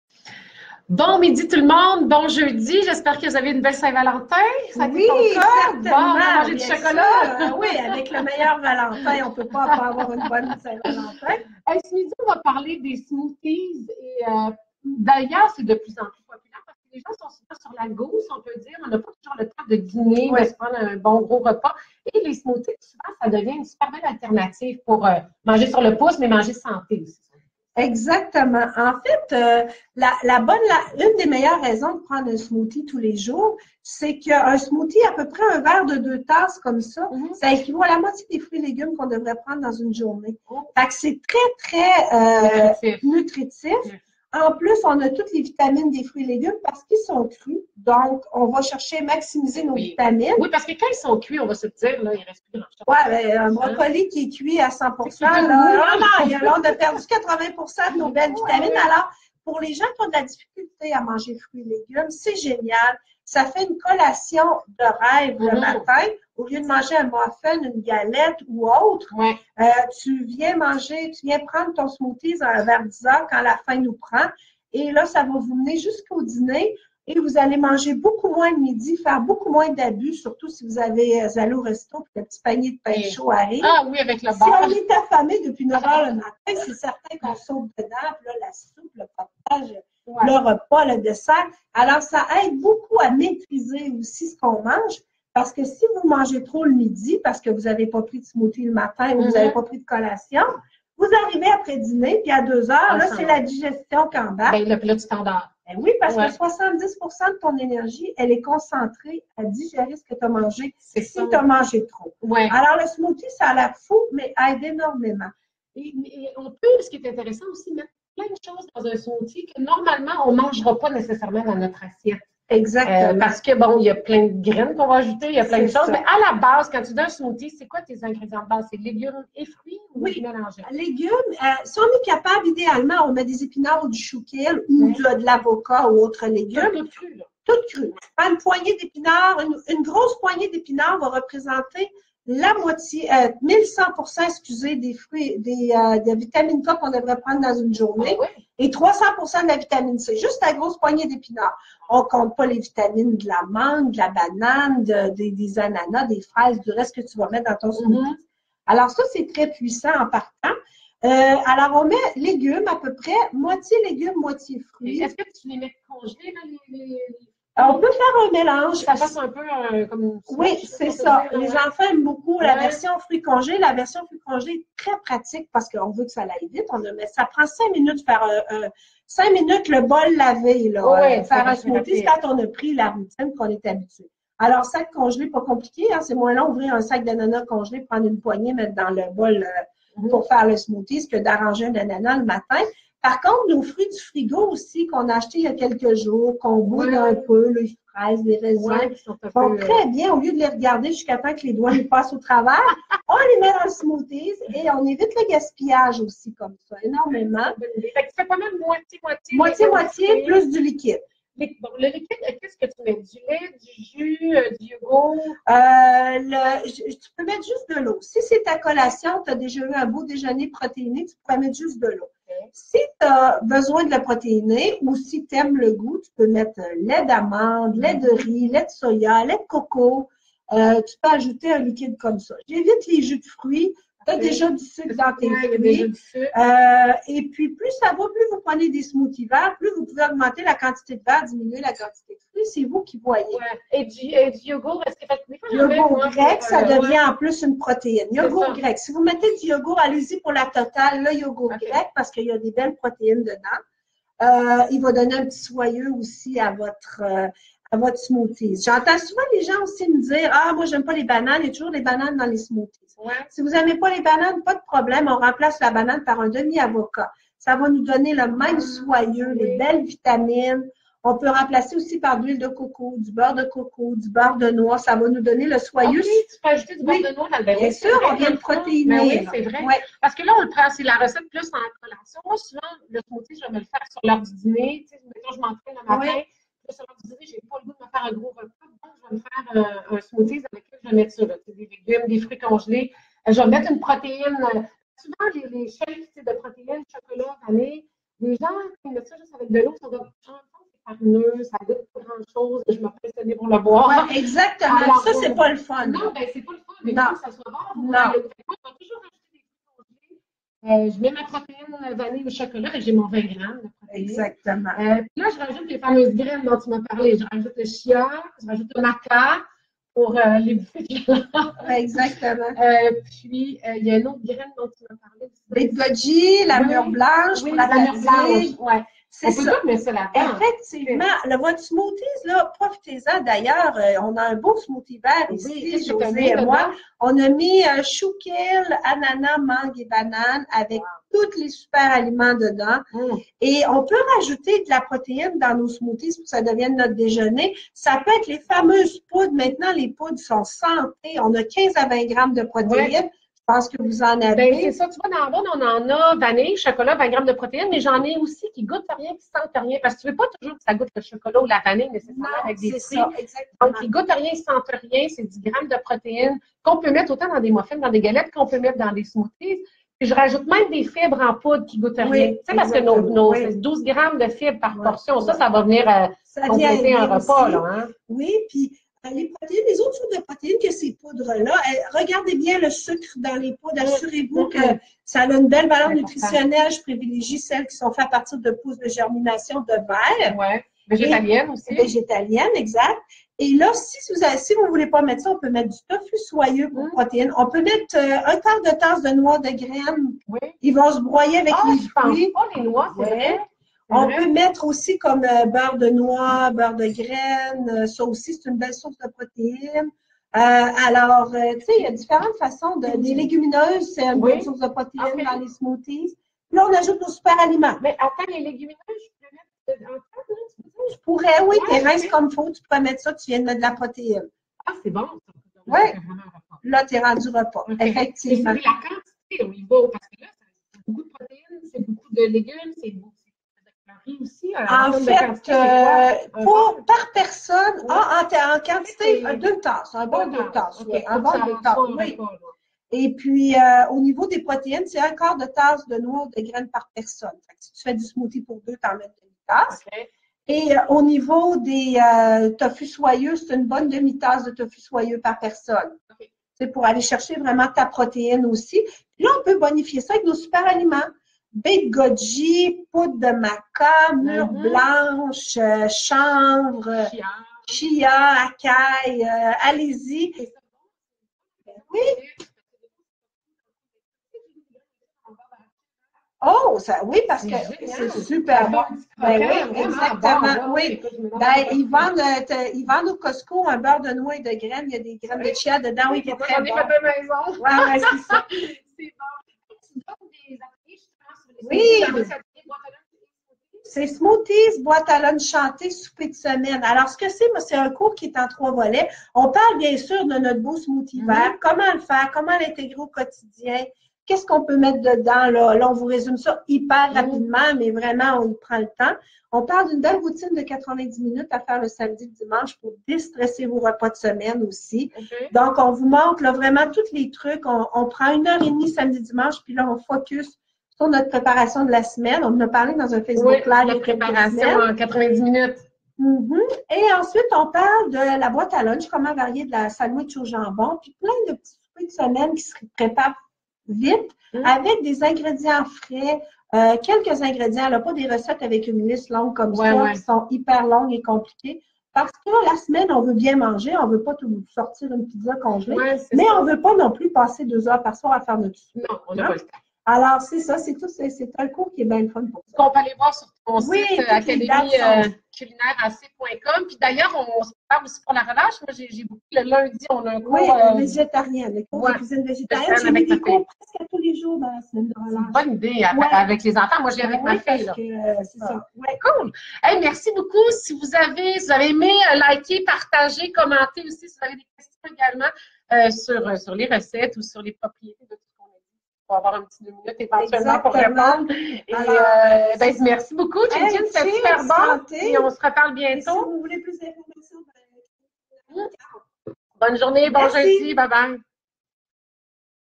"nutrition" les smoothies avec Annie Caron, coach en alimentation vivante
Les avantages des smoothies et idées de recettes avec Annie Caron coach en alimentation vivante.